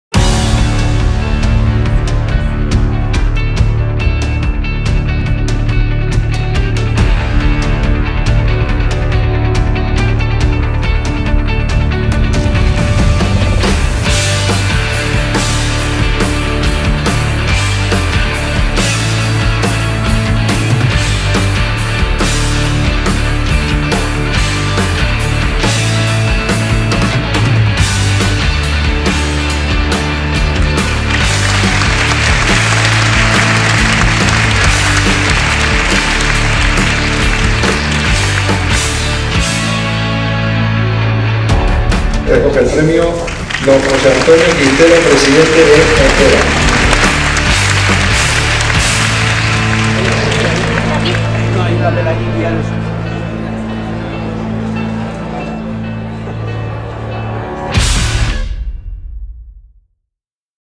Premio Nacional a la Excelencia en la Inclusión y la Prevención - Envera
Envera recibe el Premio Nacional a la Excelencia en la Inclusión y la Prevención de Riesgos Laborales para Personas con Discapacidad de la Fundación Alares.

Un premio "de todos los trabajadores con discapacidad de Envera que nos enseñan cada día que son los mejores profesionales y que, con los apoyos necesarios, todos podemos ser los mejores en algo".

El premio fue entregado a José Antonio Quintero, presidente de Envera, e Inmaculada Igualada, en representación de todos los trabajadores de Envera, por la Secretaria de Estado de Servicios Sociales del Ministerio de Sanidad, Consumo y Bienestar Social y el director general de Trabajo de la Comunidad de Madrid.

Enhorabuena a todos por este premio a los Centros Especiales de Empleo de Envera en Madrid, Colmenar Viejo, Barcelona, Málaga, Tenerife y Gran Canaria.